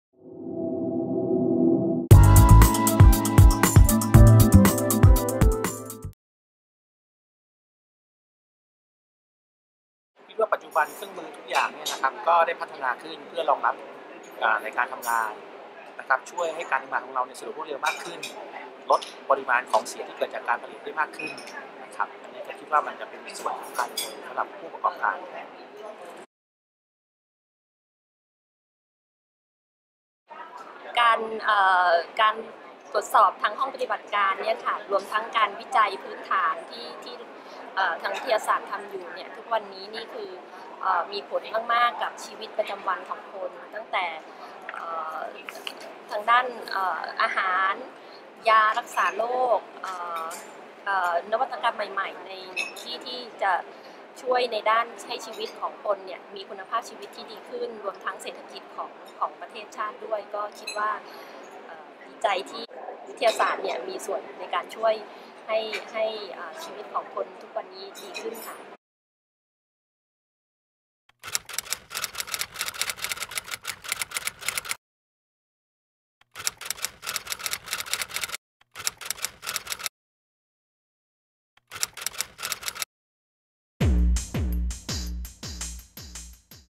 Laboratory technology, Chemical and Science processing
Question:
In an exhibitor’s vision, what are Laboratory technology, Chemical and Science processing as a part of daily living?

Interviewee's name:
Mr. Wutti Techakasembundit
Regional Sales & Marketing Manager
Ohaus Indochina

Ms. Pathanporn Prakobwanichkul
Marketing & Customer Service Manager
GibThai Co., Ltd.